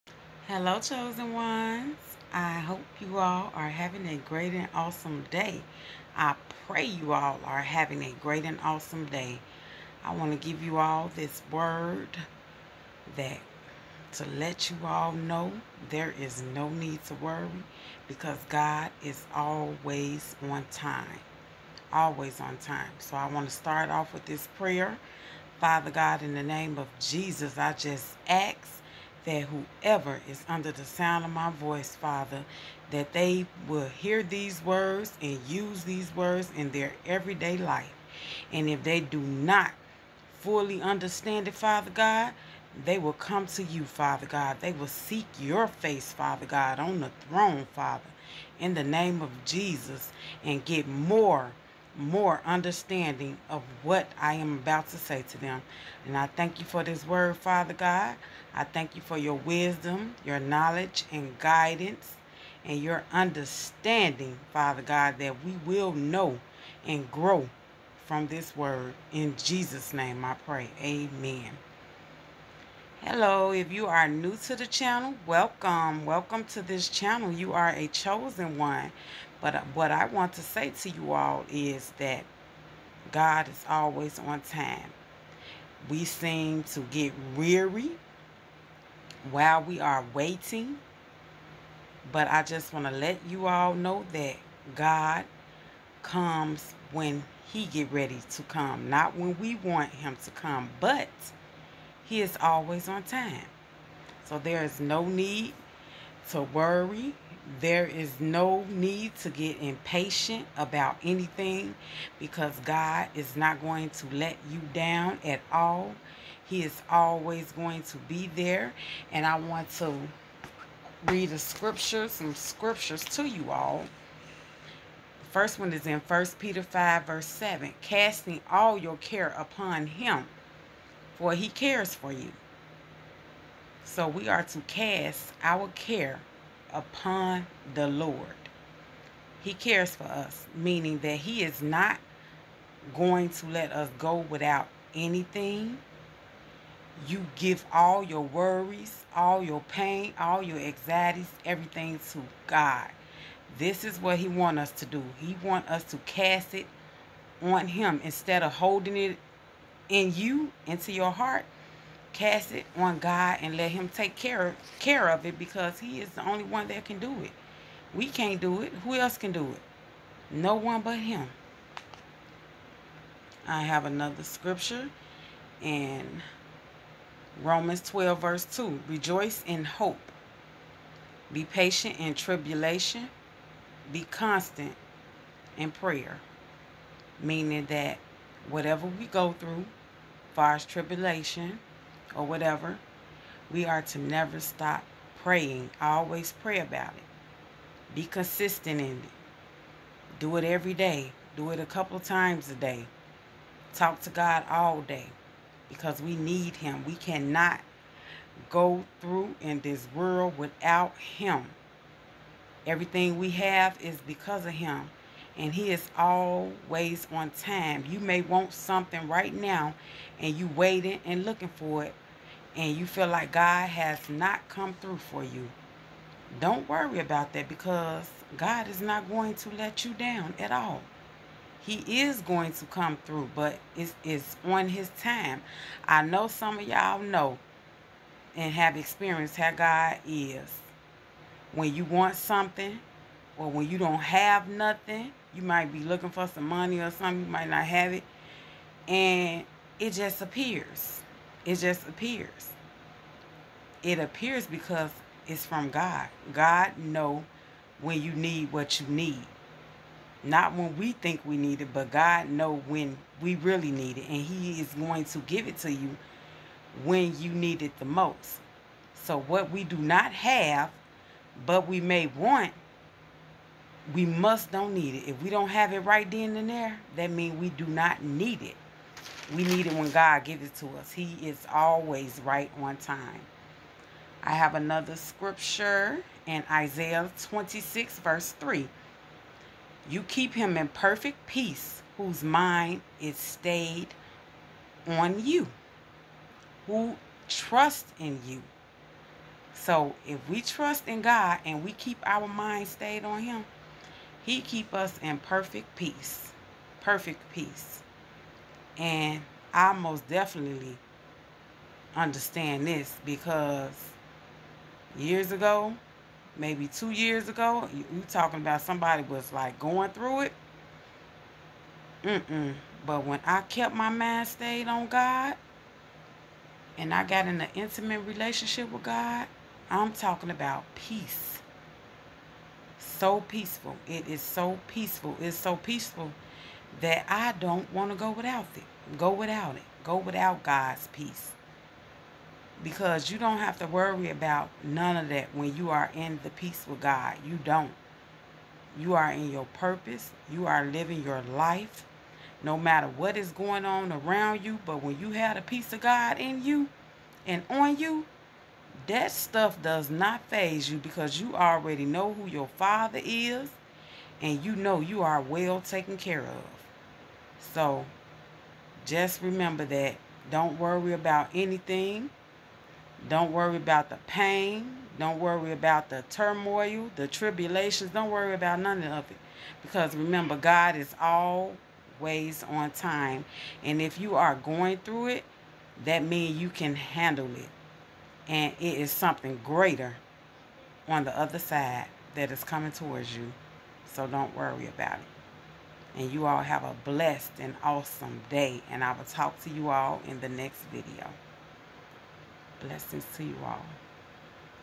GOD IS ALWAYS RIGHT ON TIME
Hello Chosen Ones!

We must know without a doubt that God is ALWAYS on time and he will never let us down.

Scriptures to reference

1 Peter 5:7
Romans 12:2
Isaiah 26:3

WEBSITES

latoyatwofford@chosenonesministries

info@chosenonesministries